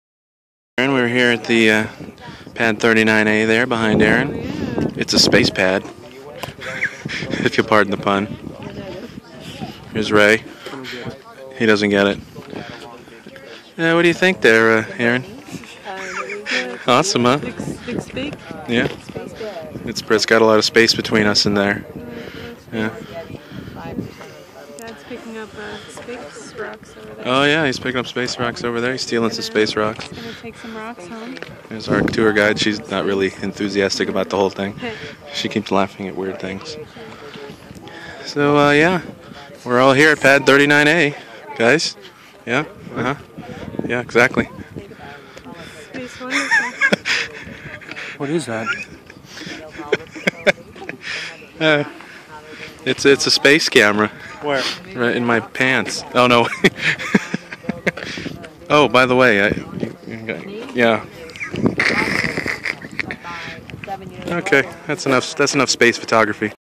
Launch pad 39a Kennedy Space Center
The fun continues at launch pad 39A Kennedy Space Center. Enjoying some space comedy!